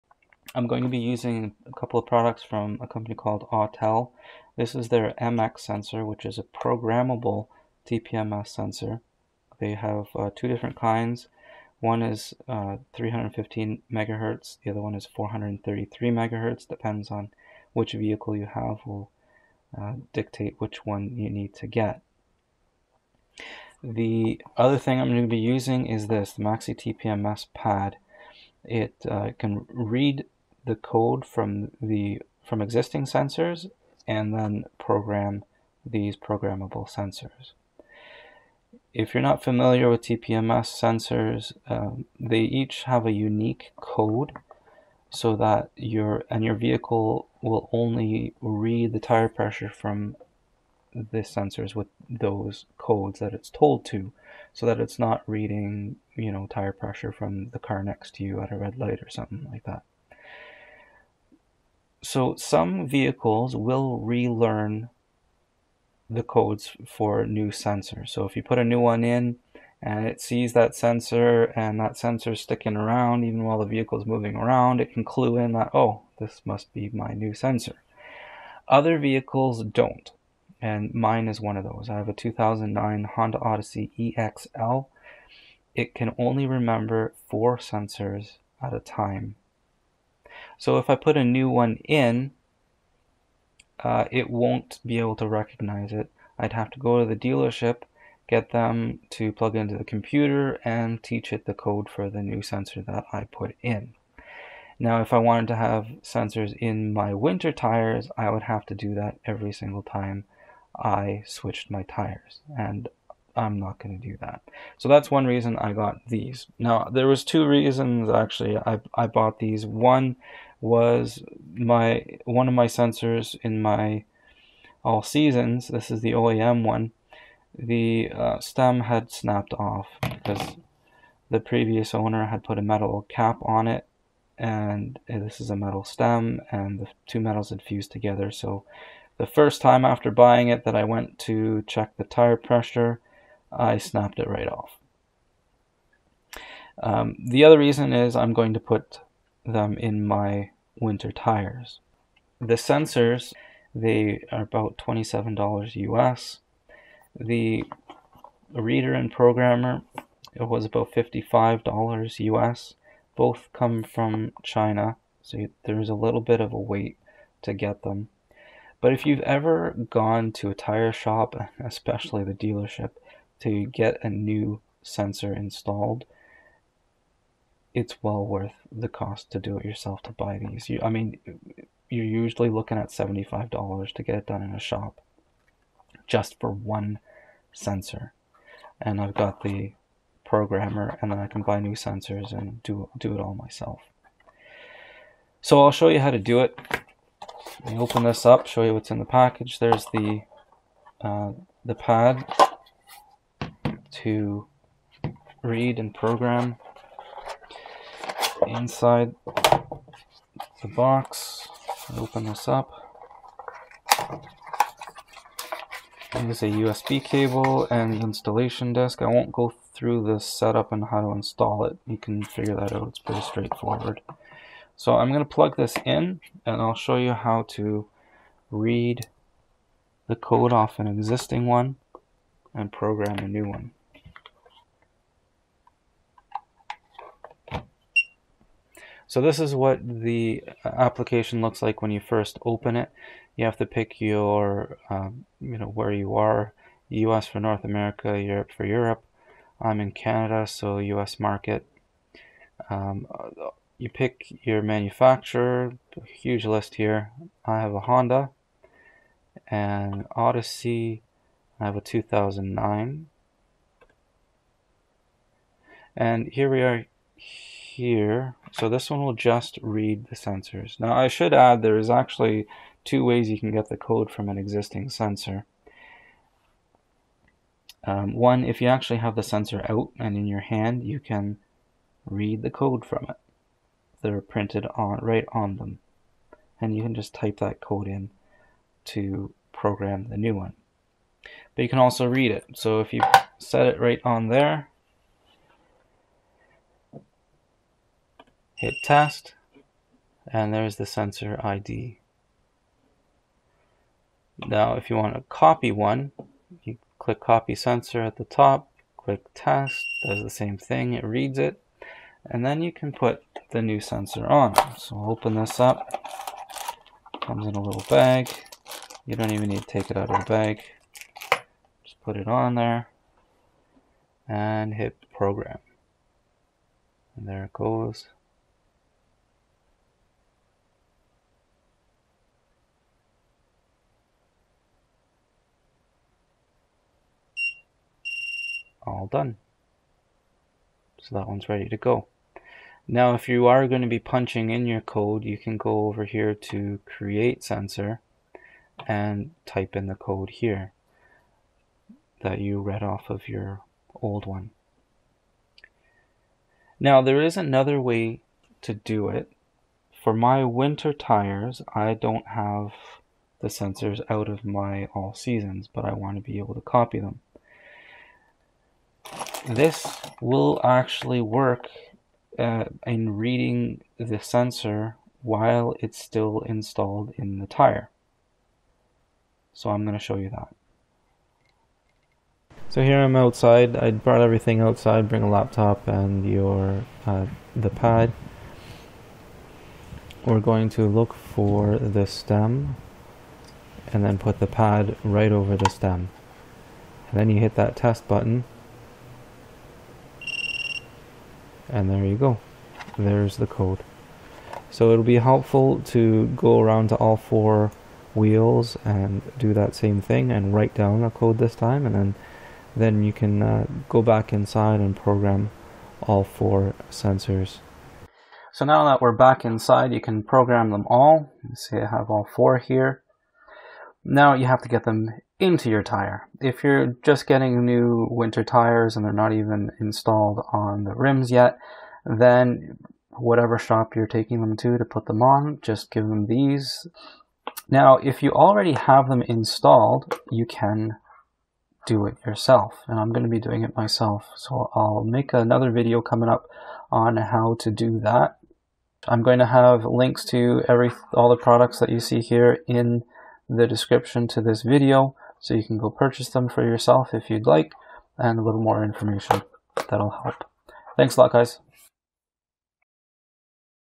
Replacement TPMS Sensors for Cheap!
For under $200, buy all 4 cloneable TPMS sensors to avoid having to visit the dealer to relearn new sensors (for vehicles that don't automatically learn new sensors).

If this has helped you, please consider donating. Any amount is helpful.

When you clone your TPMS sensors, you vehicle doesn't even know they were replaced! Use this if you are replacing a broken TPMS sensor, or if you want to install new ones.

This is also the perfect solution for adding a new set of sensors to your winter tires.

I'm doing this for my 2009 Honda Odyssey EX-L, but the principles apply to any supported vehicle.

Subaru owners have reported trouble reading the sensors while installed in the tires. If you have a Subaru, you may have to open your tires and physically read the code.

Since I posted this video, Autel has released a new sensor that works on both 315MHz and 433MHz called the "1-Sensor", so you don't need to worry about which frequency your vehicle works on.

To figure out if these sensors will work with your vehicle, go to this page, click on "Downloads" and download the document called "TS508_V6.19_FunctionList_" that ends with the two-letter code for the global market you are in ("US" for North America)

That spreadsheet will also tell you whether your vehicle automatically learns new TPMS sensors. If it says "A", then you do not need programmable sensors at all. Just buy any compatible sensor that works on the right frequency for your vehicle.

You can order the programmable MX sensors online:

eBay (shipped from China)
Amazon.com (shipped from US)

And the programmer:
Autel MaxiTPMS PAD (eBay - from China)

0:00 Intro
3:14 PAD box opening
3:55 Read existing sensor
5:03 Copy sensor
6:21 Read installed sensor
6:57 Conclusion